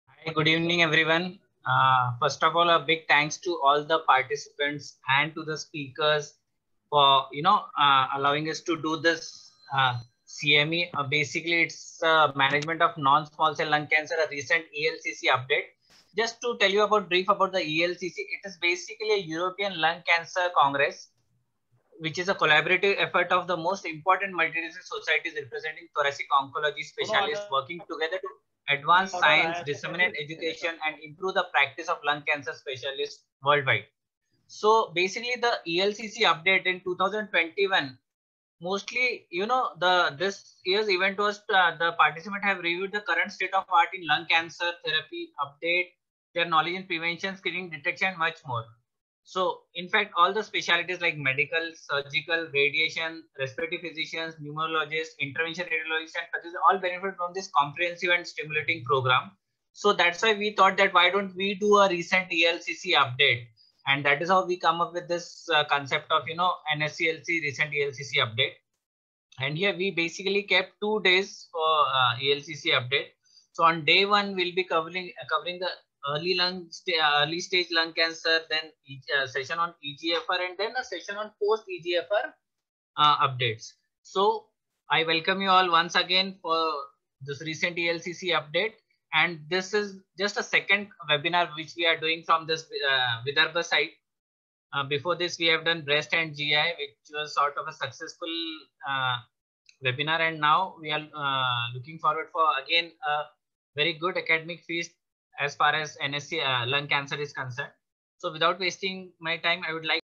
Management of Non Small Cell Lung Cancer | Dr Amol Dongre | Welcome & Introduction
Cancer Research And Statistic Foundation organized a webinar "Management of Non-Small Cell Lung Cancer" with the support of "Institute of Pulmonology Medical Research & Development" & "Sunrise Oncology Centre".

Event Managed By:
Horizon Conference Management